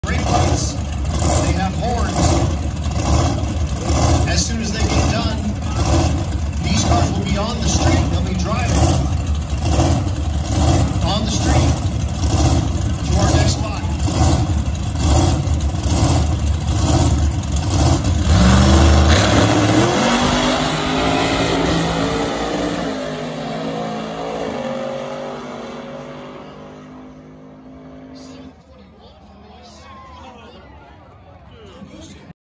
Camaro VS Mustang Rocky Mountain Race Week 2020 Tulsa, OK Tulsa Raceway
Two sweet rides at the Tulsa Race Way in Tulsa Oklahoma September 13 2020 both ran a 7.41 (which is insane) Both apart of Rocky Mountain Race Week 2020